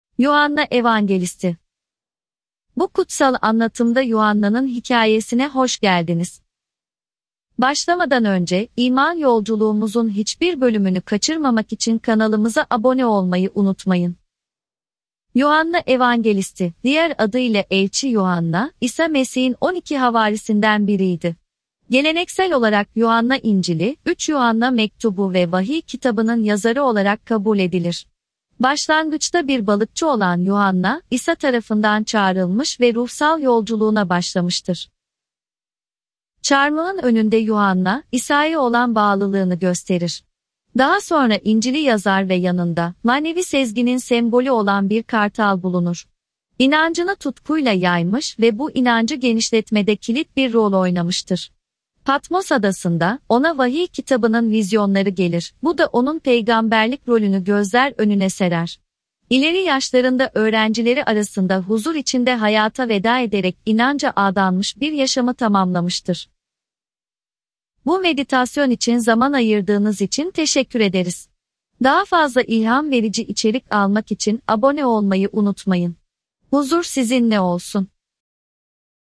Yuhanna Evangelist, Yuhanna Havari
Yuhanna Evangelist, diğer adıyla Yuhanna Havari, İsa Mesih’in on iki havarisinden biriydi. Geleneksel olarak Yuhanna İncili, üç Yuhanna Mektubu ve Vahiy Kitabı’nın yazarı olarak kabul edilir.

Yuhanna, Celile Gölü kıyısında balıkçılık yapıyordu. Bir gün İsa onu havarisi olarak çağırdı, bu çağrı Yuhanna'nın ruhsal yolculuğunun başlangıcı oldu.

İsa’nın çarmıha gerilişi sırasında Yuhanna, çarmıhın dibinde durarak üzüntüsünü ve bağlılığını gösterdi, bu da İsa ile olan derin bağını vurguladı.

Yuhanna İncili’ni, yanında manevi sezginin sembolü olan bir kartal ile, doğal ışıkla aydınlanan sade bir odada yazar.

İlk Kilise döneminde kalabalığa tutkuyla vaaz veren Yuhanna, Hristiyan inancının yayılmasında önemli bir rol oynar.

Patmos Adası’nda, Vahiy Kitabı’nın vizyonlarını alarak peygamberlik rolünü ve o anın manevi önemini gözler önüne serer.

İleri yaşlarında öğrencilerinin arasında huzur içinde hayata veda eden Yuhanna, Hristiyan inancına adanmış bir yaşamı noktalar.